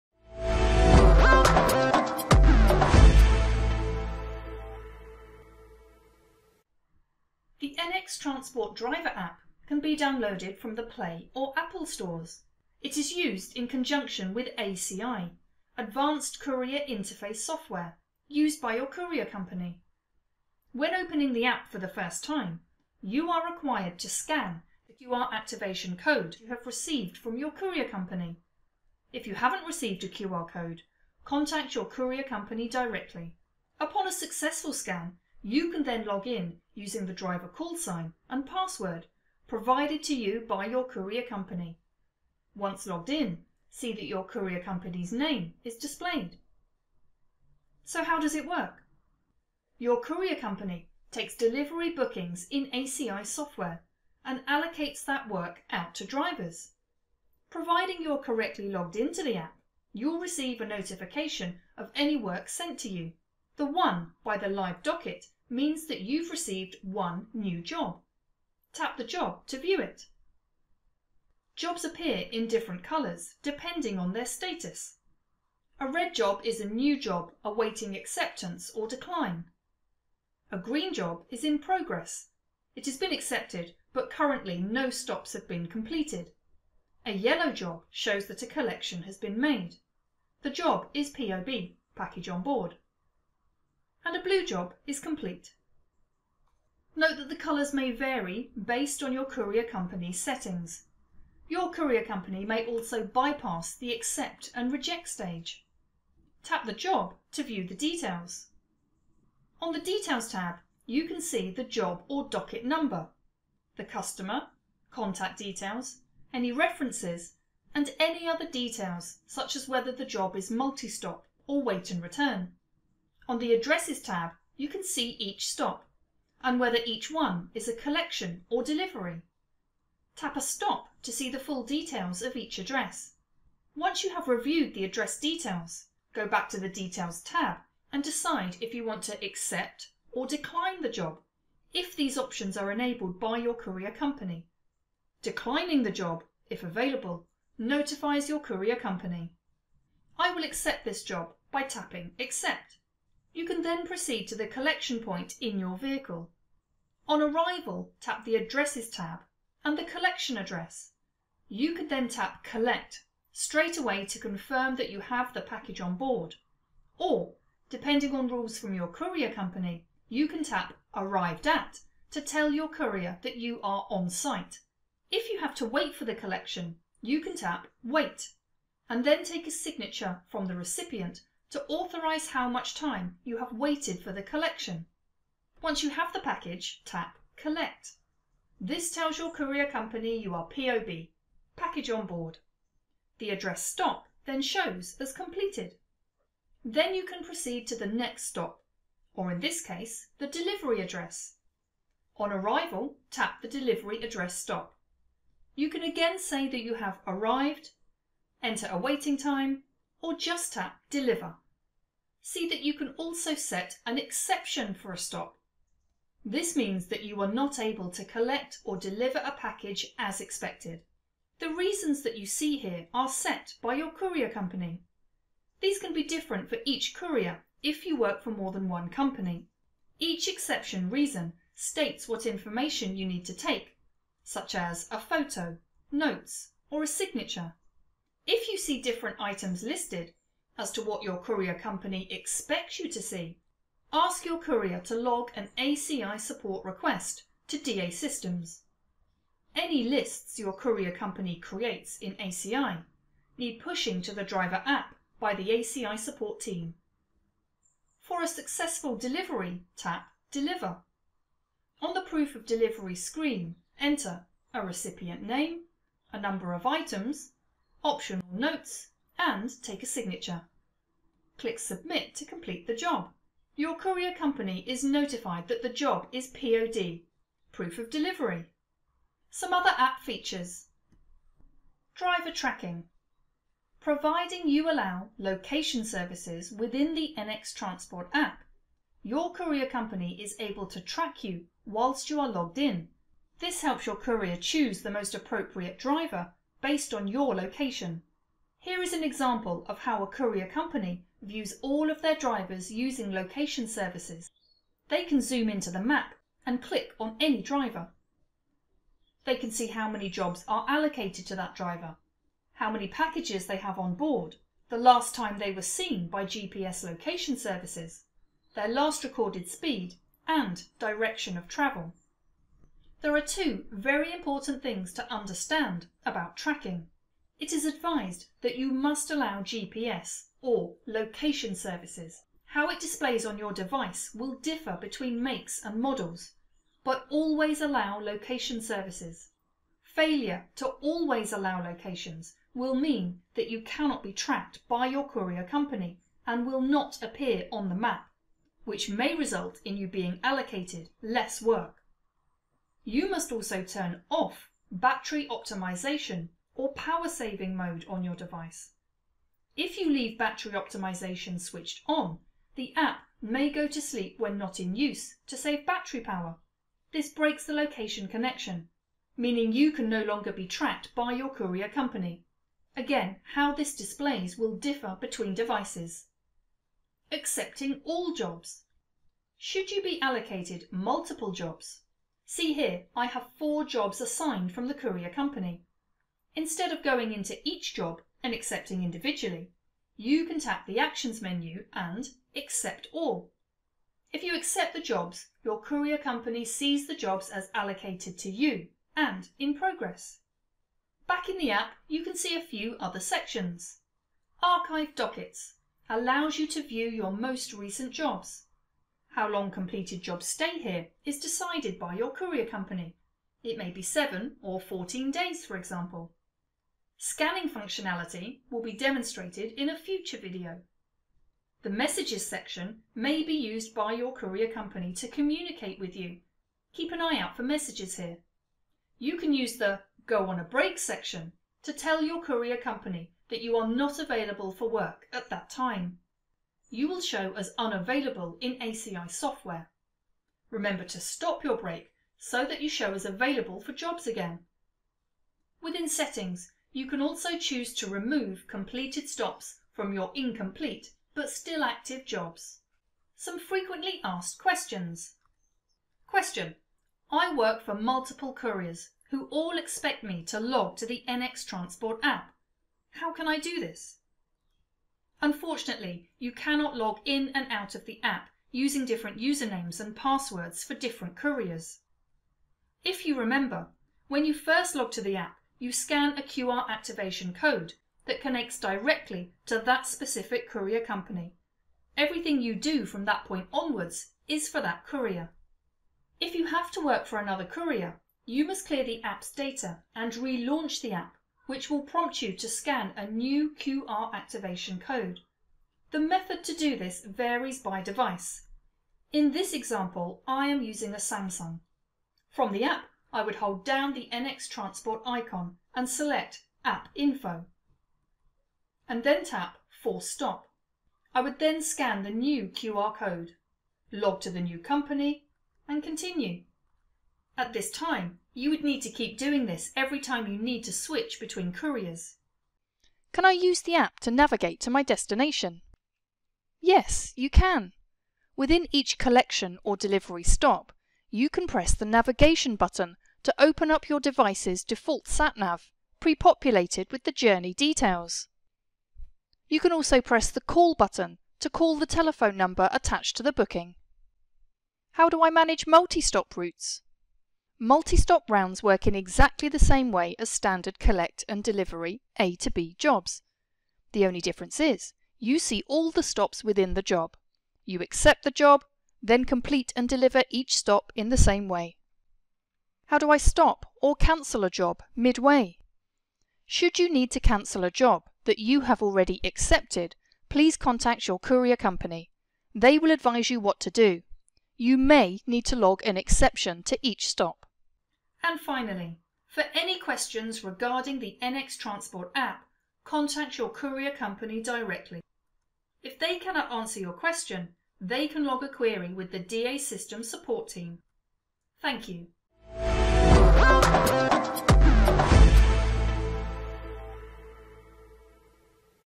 NX Transport
This video explains best practices to NX Transport app Drivers.
It describes how to set up the app, receive, accept and decline jobs.
It also looks at Fleet Tracking and important actions the driver should take in relation to 'Location Services' and 'Battery Optimisation' device settings.